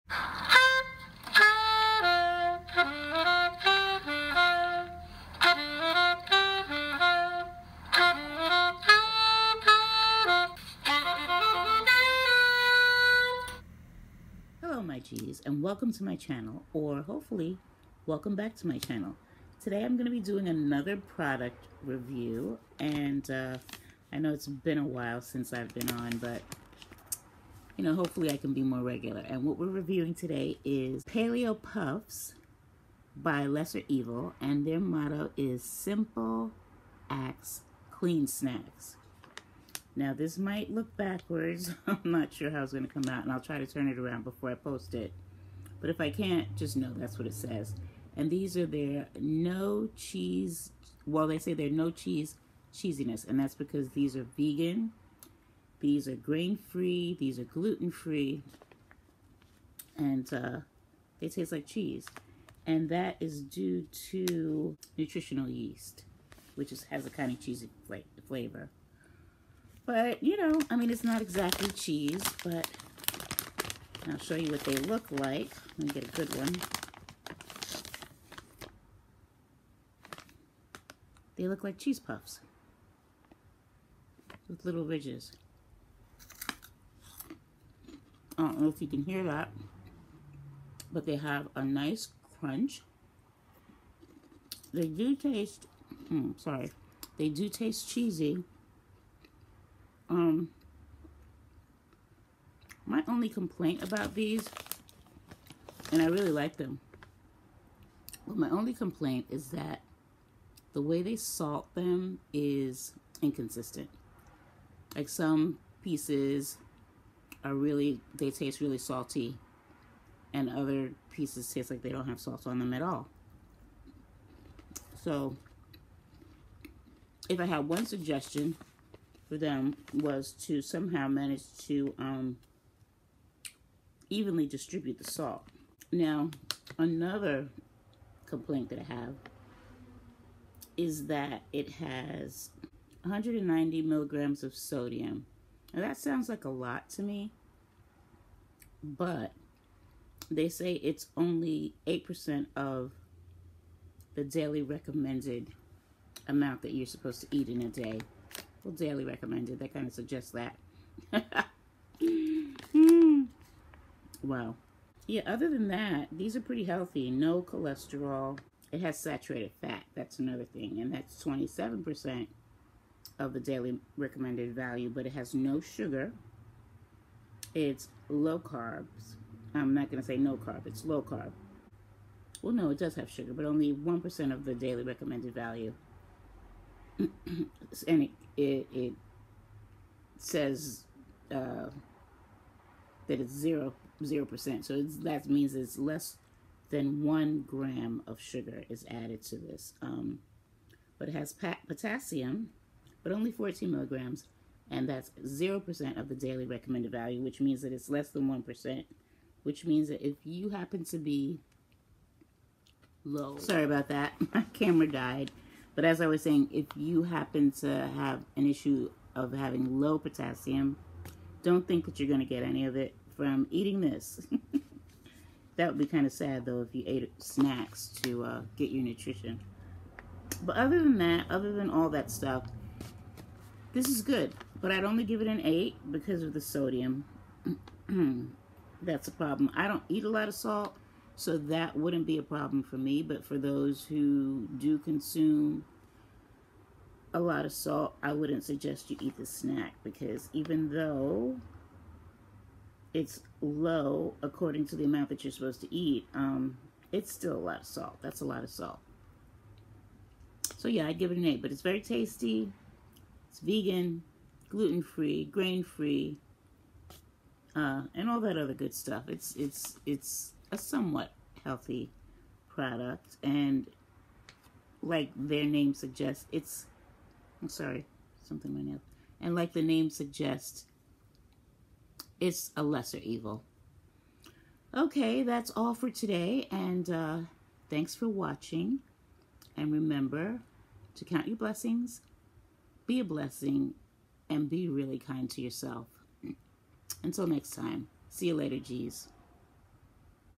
Let’s Review Paleo Puffs
Find out what I think of this gluten free snack, Paleo Puffs by Lesser Evil.